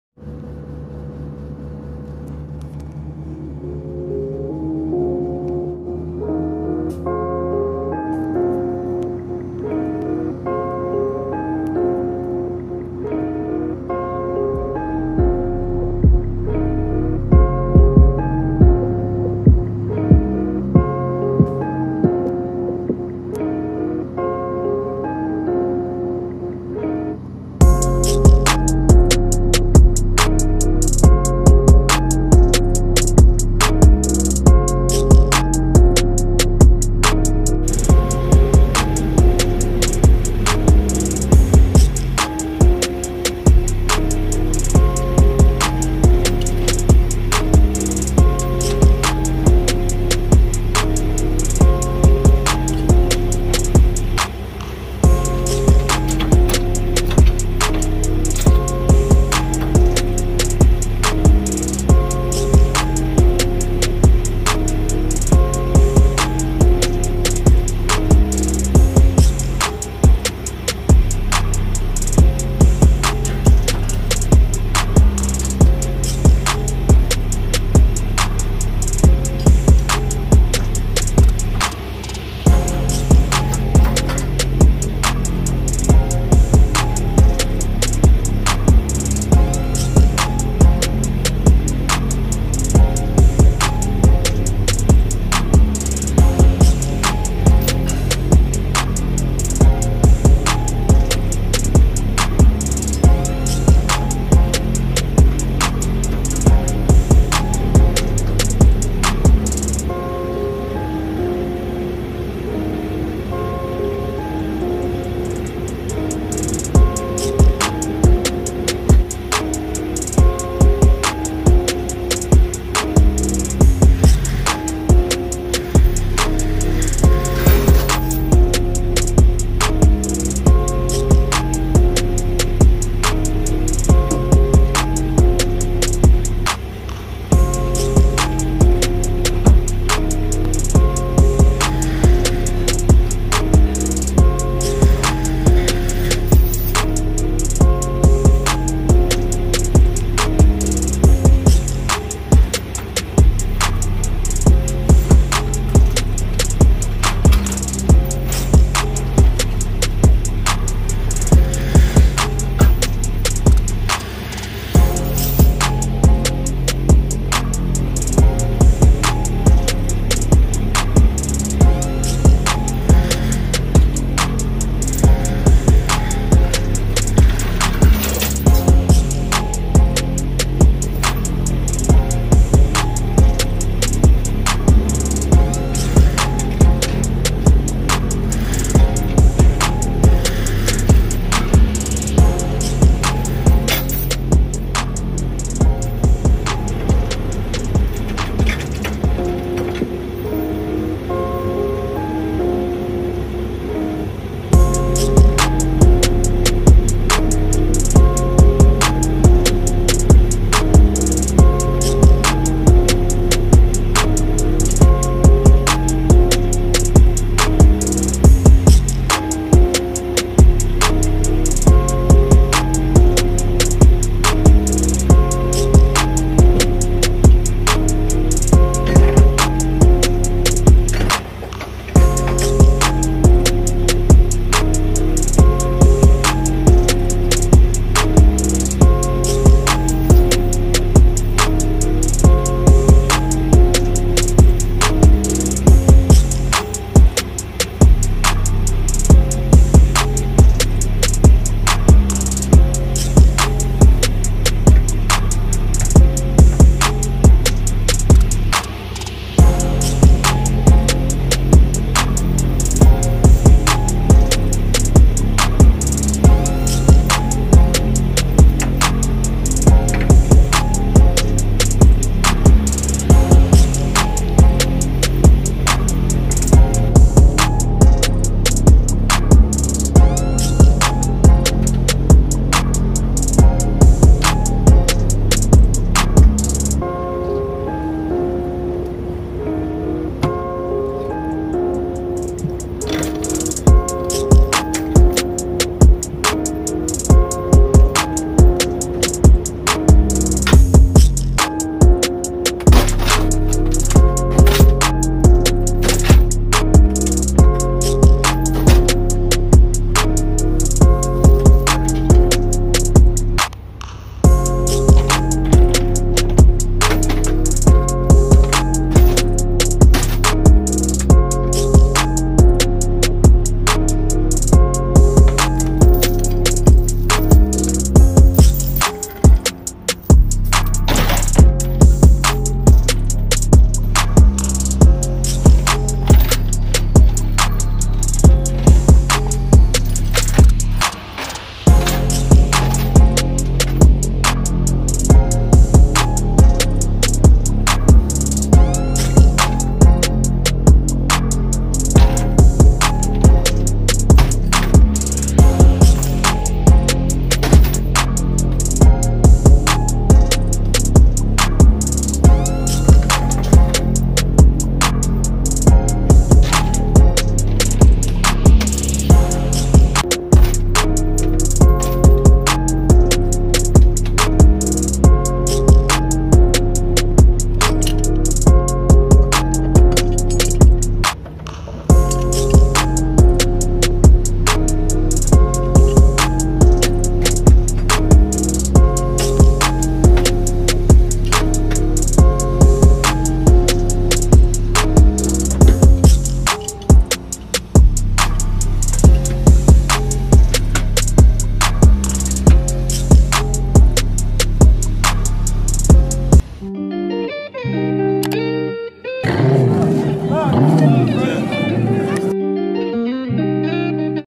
HAULING STEEL BEAMS
FLATBED TRUCK DRIVER HAULING BIG BEAMS MADE OF STEEL AS A ROOKIE TRUCK DRIVER